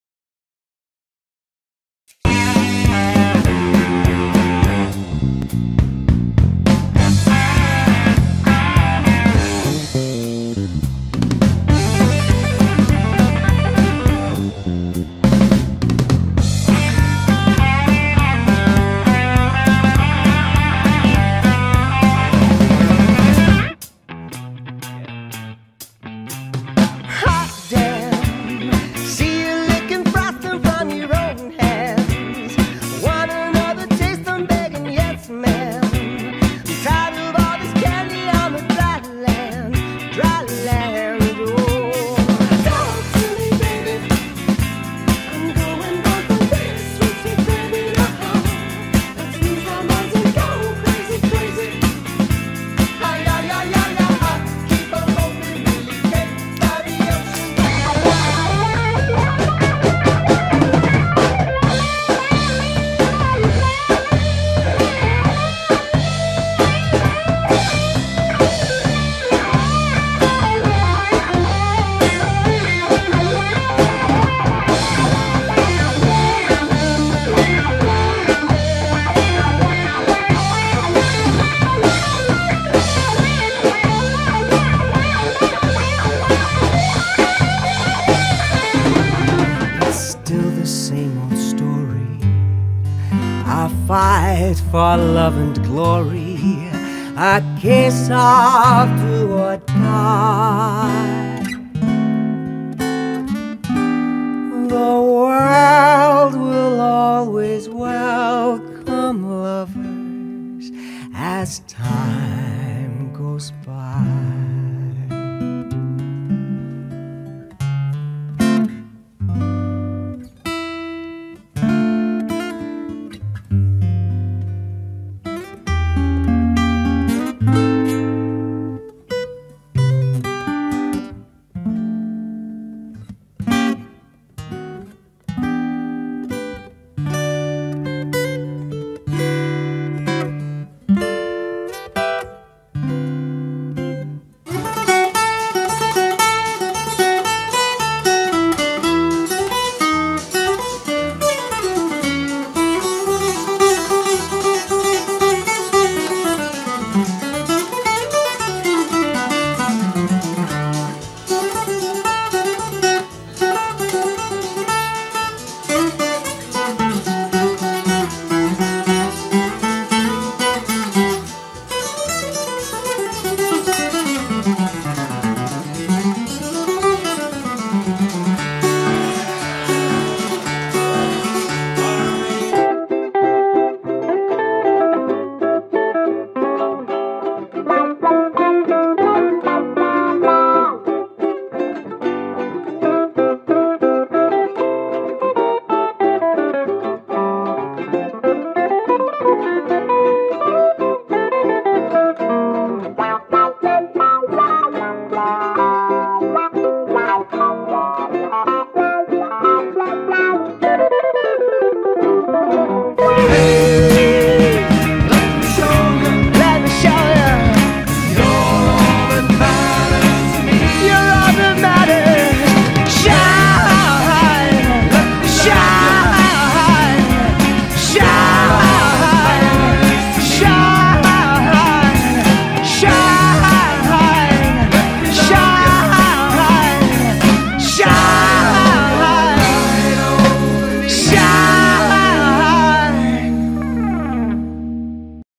Rob Shaw - Guitarist Showreel
Showreel for Manchester/North West based guitarist Rob Shaw.
Available for dep and function band work and sessions.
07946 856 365